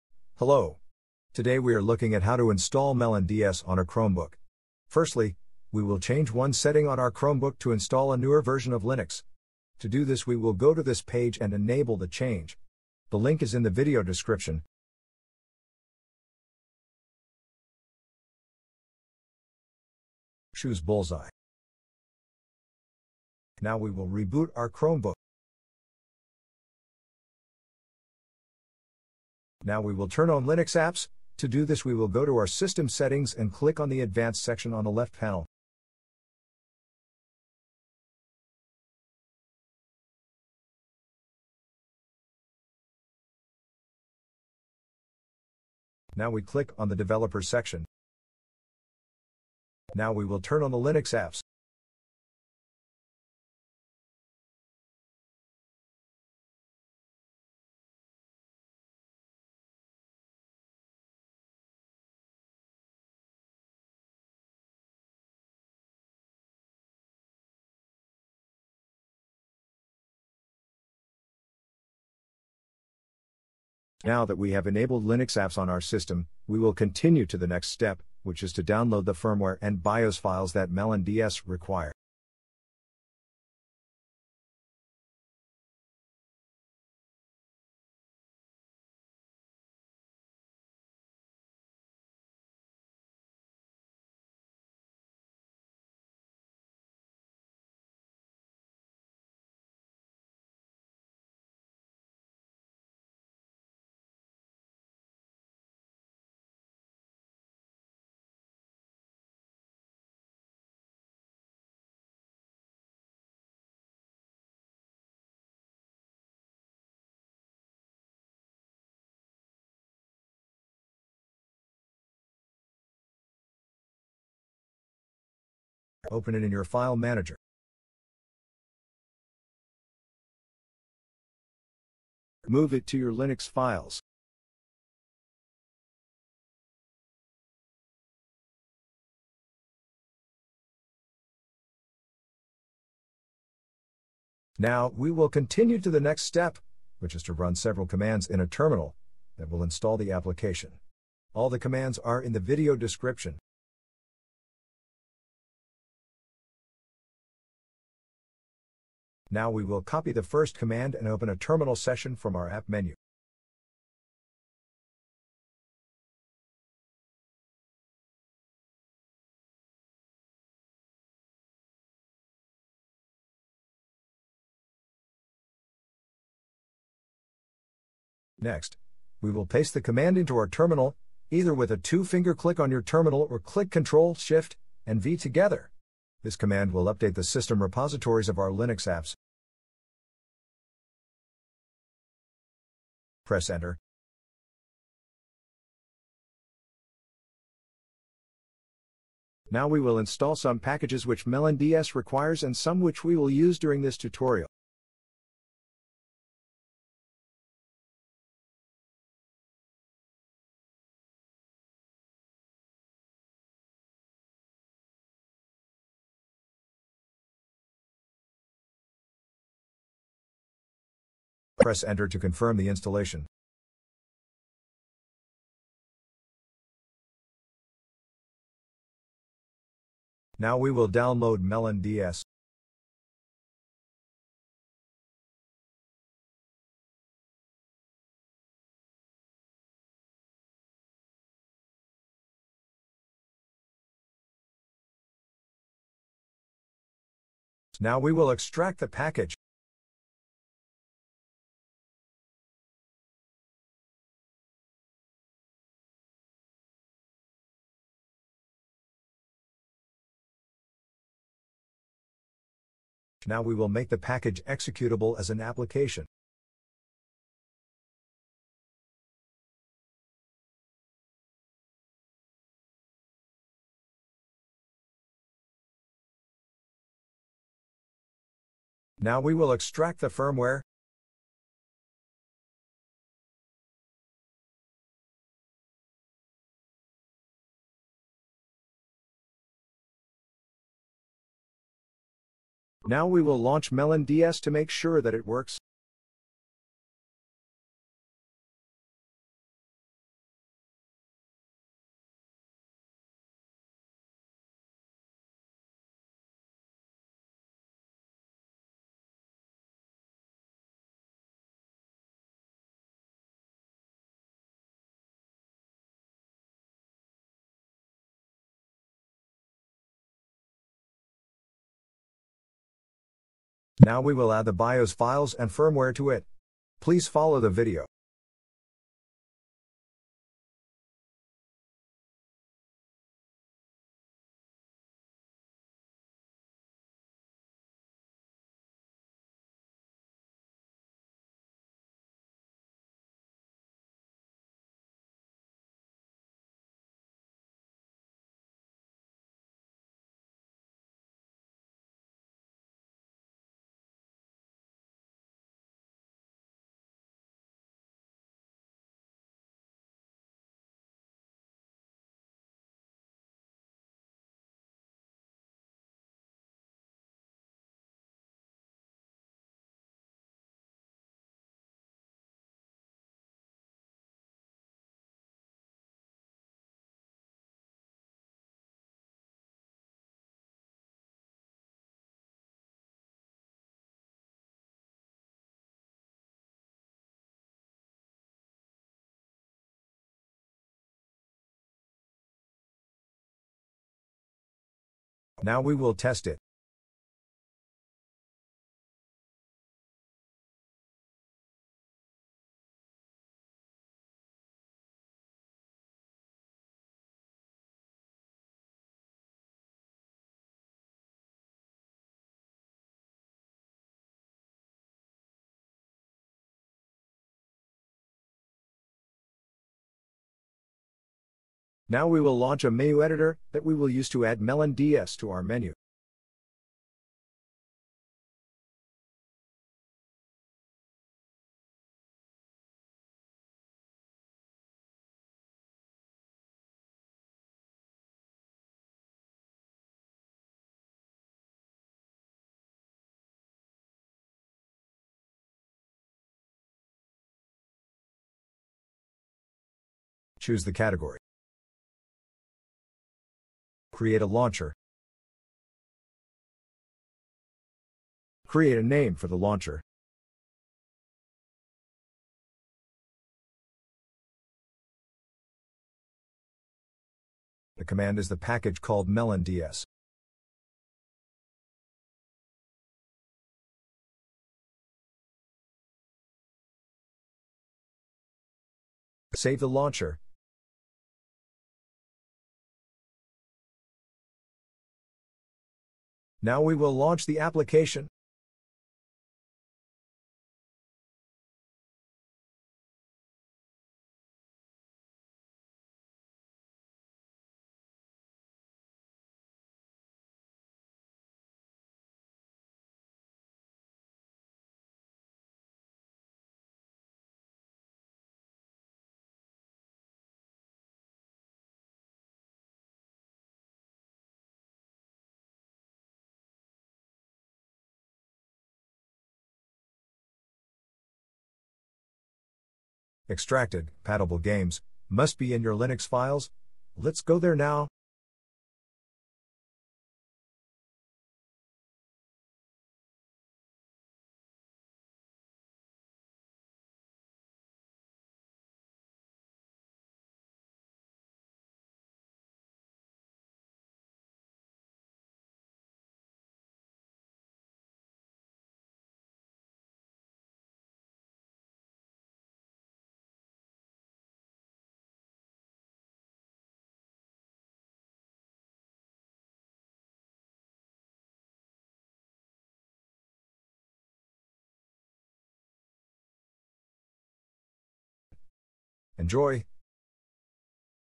How to install MelonDS on a Chromebook - Updated Tutorial
In this video, we are looking at how to install MelonDS on a Chromebook.

Please take note that controllers or joysticks sadly doesnt work.